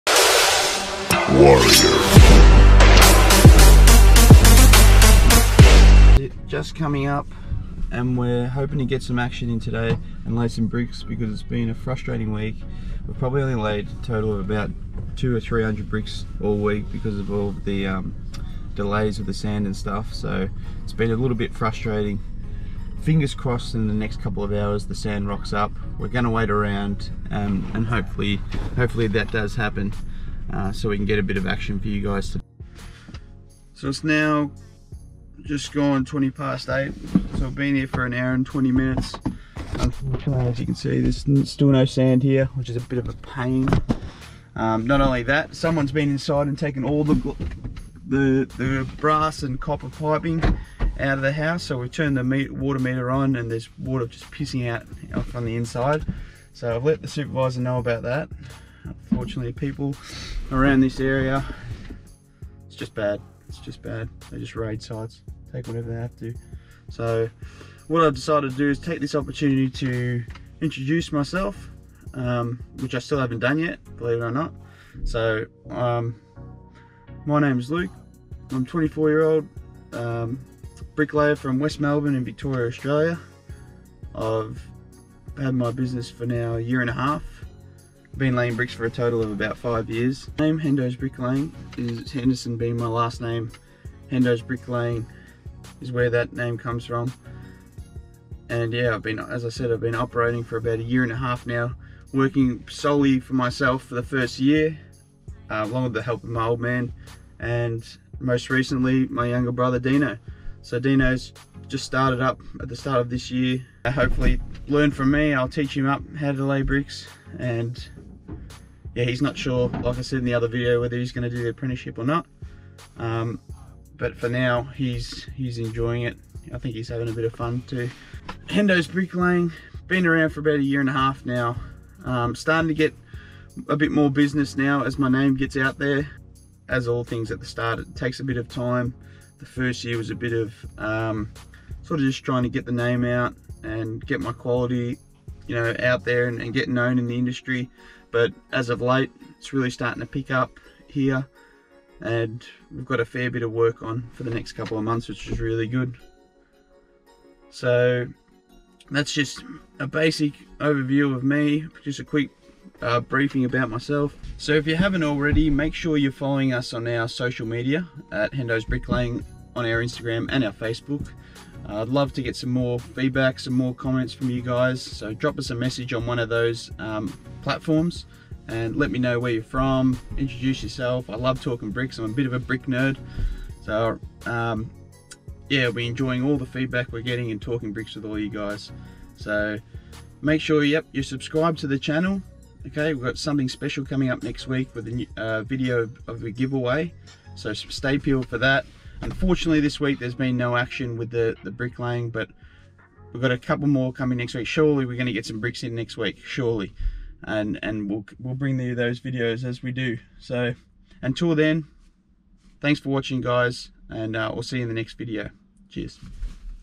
Hendos Bricklaying Introduction
So I haven't formally introduced both myself and the channel!!

Today as we have had a pretty quiet week with all the Interruptions with the corona virus pandemic, I thought it would be the perfect time to so.

Happy Laying, Hendo!